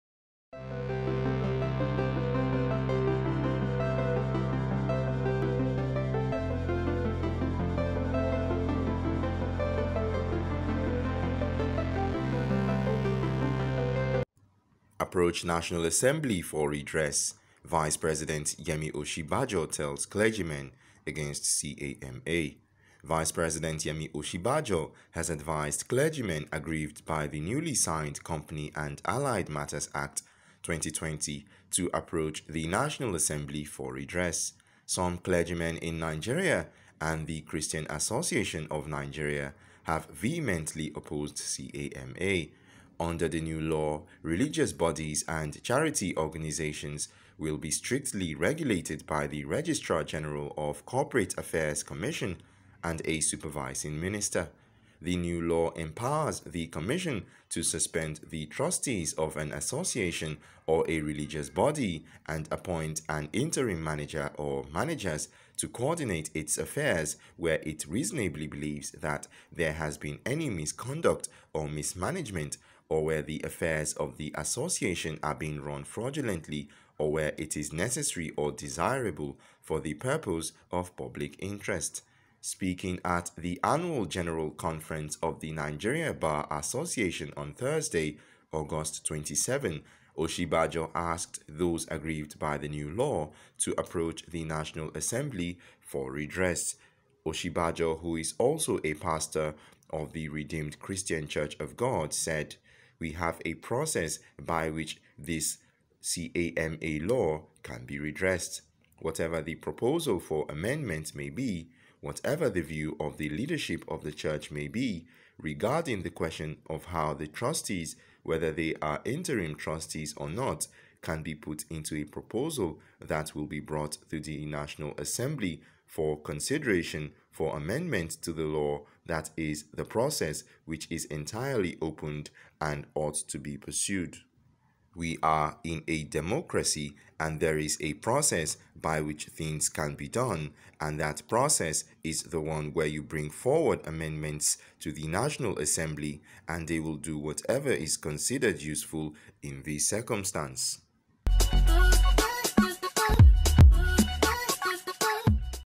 Approach National Assembly for redress- Vice President Yemi Osinbajo tells clergymen against CAMA
Vice-President Yemi Osinbajo has advised clergymen aggrieved by the newly signed Company and Allied Matters Act (CAMA) 2020, to approach the National Assembly for redress.

Some clergymen in Nigeria and the Christain Association of Nigeria CAN have vehemently opposed CAMA. Under the new law, religious bodies and charity organizations will be strictly regulated by the registrar-general of Corporate Affairs Commission (CAC) and a supervising minister. The new law empowers the commission to suspend the trustees of an association or a religious body and appoint an interim manager or managers to coordinate its affairs where it reasonably believes that there has been any misconduct or mismanagement, or where the affairs of the association are being run fraudulently or where it is necessary or desirable for the purpose of public interest.

Speaking at the annual general conference of the Nigeria Bar Association on Thursday, August 27, Osinbajo asked those aggrieved by the new law to approach the national assembly for redress.

Osinbajo who is also a pastor of the Redeemed Christian Church of God (RCCG), said

“We have a process by which this (CAMA law) can be redressed. Whatever the proposal for amendment may be, whatever the view of the leadership of the church may be, regarding the question of how the trustees, whether they are interim trustees or not, can be put into a proposal that will be brought to the National Assembly for consideration for amendment to the law, that is the process which is entirely opened and ought to be pursued.

We are in a democracy and there is a process by which things can be done and that process is the one where you bring forward amendments to the National Assembly and they will do whatever is considered useful in the circumstance.”